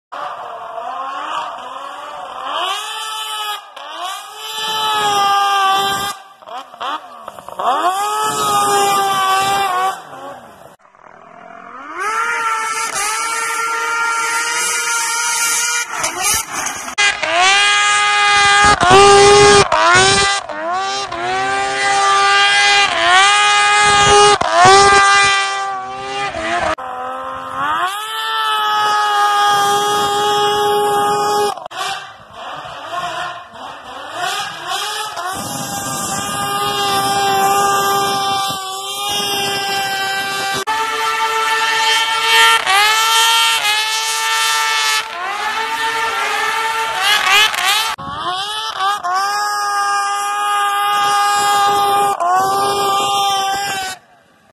vmax 700 triple soundclip compilation ( snostuff can )
few short clips i had on my phone.